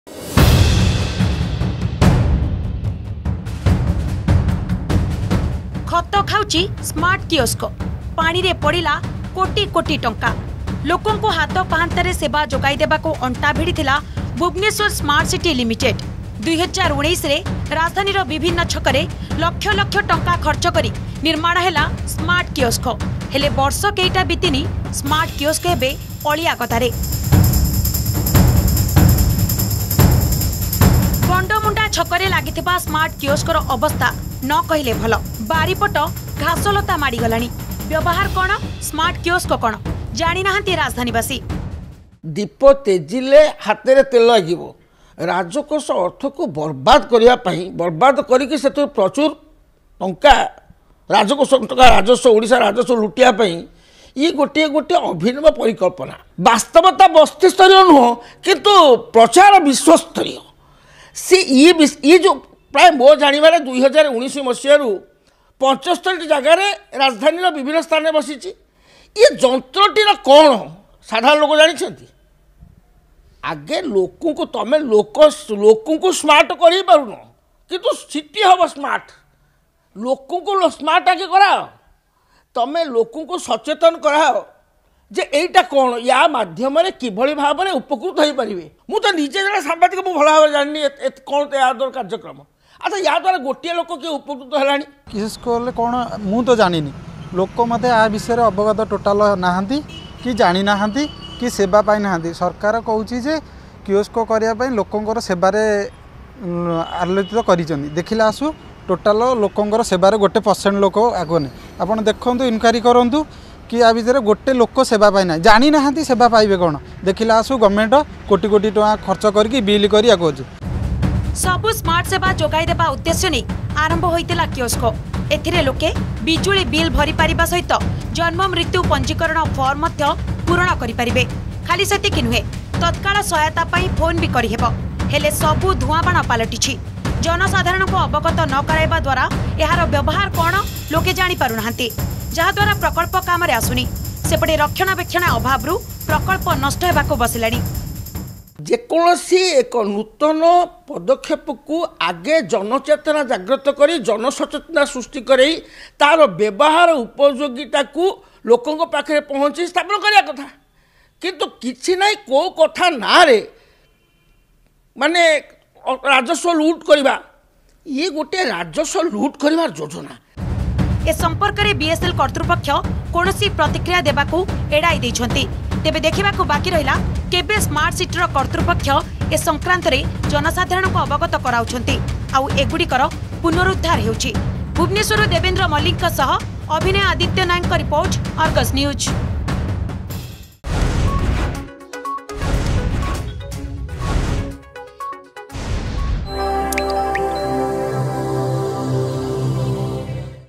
Bhubaneswar: Smart kiosk are Getting deteriorated Without Coming in any Use
More than 50 smart kiosks have been installed in key areas of Bhubaneswar not just one or two. Hoping the kiosk will provide all the smart services. But Residents of the capital do not use Kiosk because they do not know the use. As a result, the Smart kiosk has fallen into disrepair.